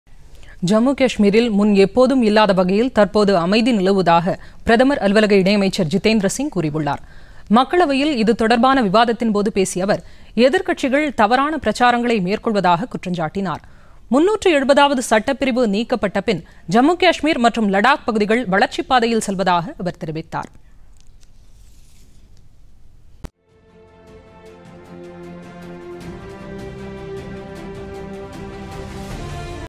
ஜம்மு கஷ்மீரில் அமைதி நிலவுகிறது - இணையமைச்சர் ஜித்தேந்திரசிங்
ஜம்மு கஷ்மீரில் முன் எப்போதும் இல்லாத அளவில் தற்போது அமைதி நிலவுகிறது - பிரதமர் அலுவலக இணையமைச்சர்  ஜித்தேந்திரசிங்